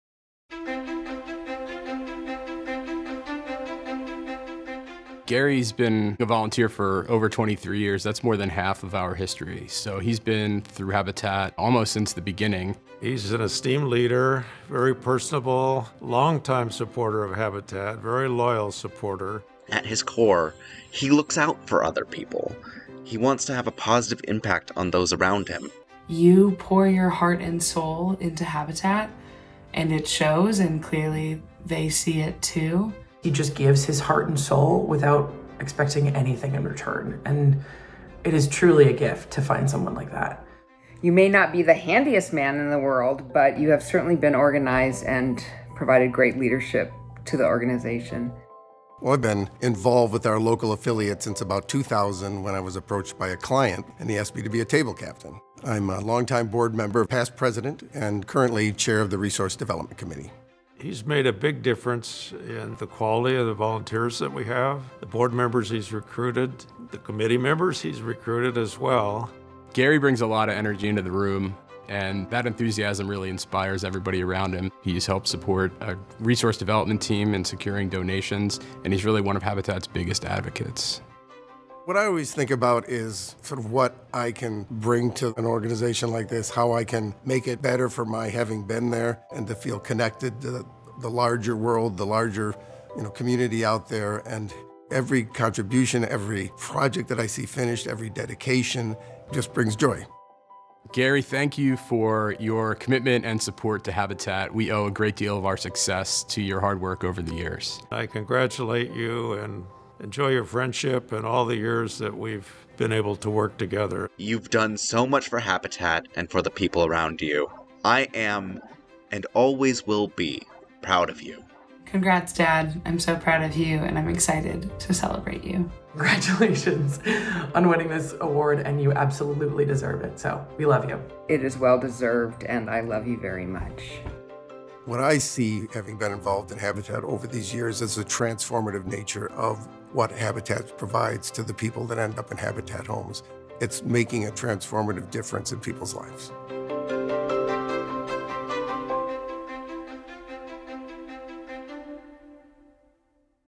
2023 Habitat for Humanity SKKC Volunteer of the Year
Join us in honoring the exceptional dedication and selfless service of our Volunteer of the Year in this heartwarming video. Witness the impact and inspiring stories that have earned this individual the esteemed recognition within the Habitat for Humanity Seattle-King and Kittitas Counties (SKKC) community.

Discover the powerful moments that highlight the Volunteer of the Year's commitment to our mission of providing safe and affordable housing to families in need. Through their unwavering passion, countless hours of service, and positive influence, they have become an invaluable member of our Habitat SKKC family.

This video showcases the Volunteer of the Year's journey, their profound impact on the lives of others, and the ripple effect of their efforts on our community. From their first hammer swing to the last nail driven, their dedication shines as a beacon of hope and inspiration.

Join us in celebrating this remarkable individual and their significant contribution to the Habitat SKKC mission. Watch the 2023 Habitat for Humanity SKKC Volunteer of the Year video to witness the power of volunteering and the difference one person can make in the lives of many.

Together, we can continue to build a better future for families in need.